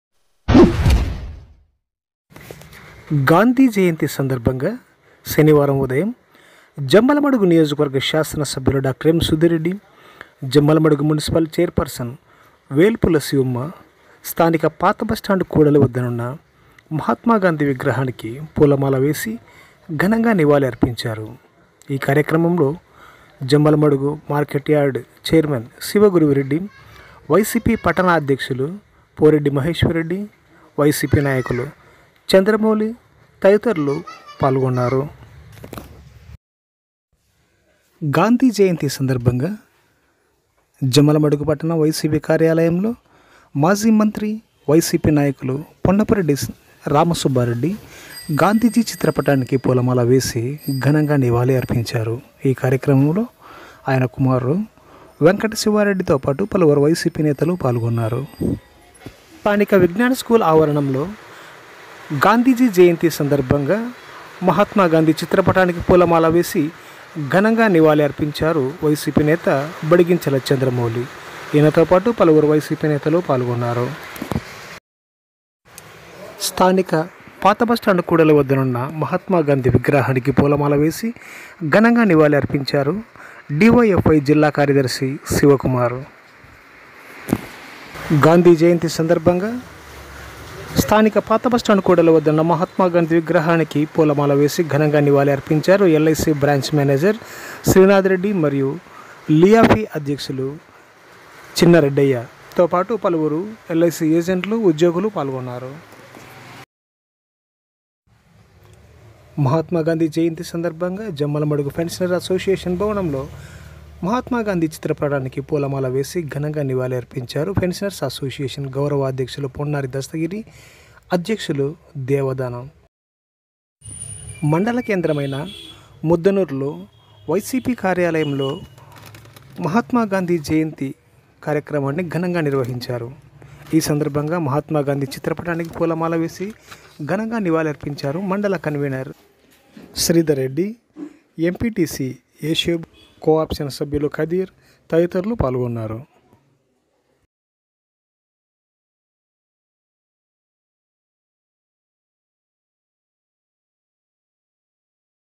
గాంధీ జయంతి సందర్భంగా ఘనంగా నివాళి అర్పించిన ఎమ్మెల్యే సుధీర్ రెడ్డి,మాజీమంత్రి పి.రామసుబ్బారెడ్డి.
For Latest News YSR KADAPA NEWS